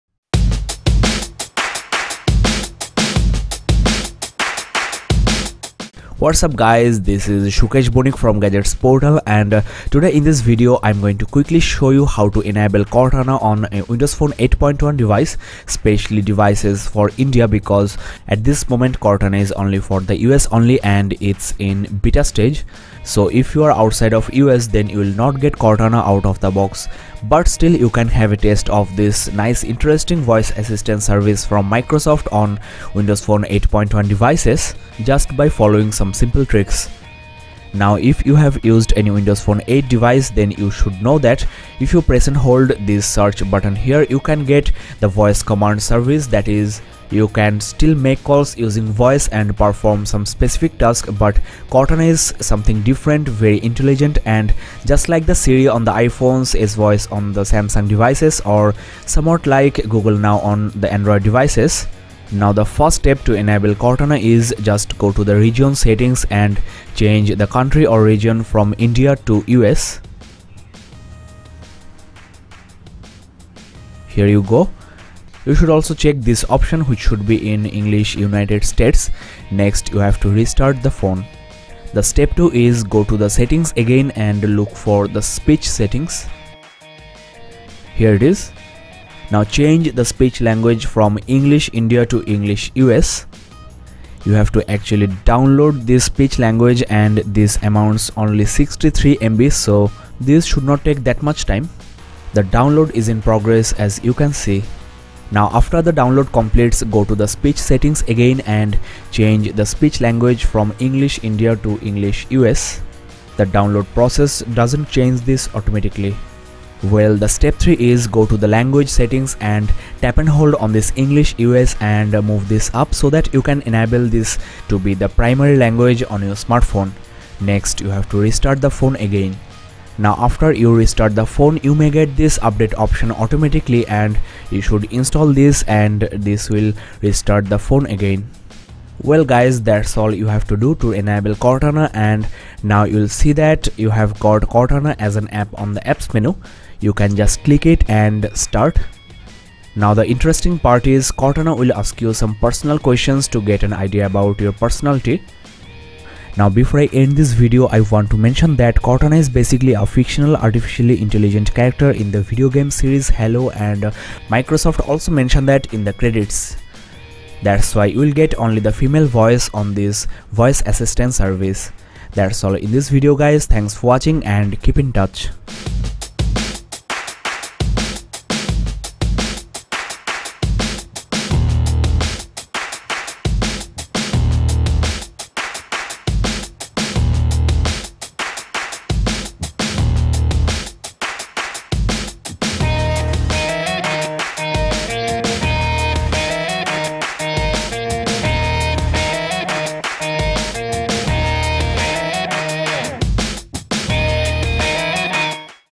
How To Enable CORTANA on a Windows Phone 8.1 device [Lumia 730/530/630]
3 simple steps to enable 'Cortana' voice assistance service on any non-US Windows Phone 8.1 device ft. Nokia Lumia 630. Cortana is just like 'Siri' on iPhones, S-voice on Samsung phones & 'Google Now' to some extent. Lumia 630 is one of the first phones to get the Microsoft Cortana out-of-the box but limited to US consumers only at this moment. This tutorial shows how to get this on Lumia 630 on any other WP 8.1 smartphone.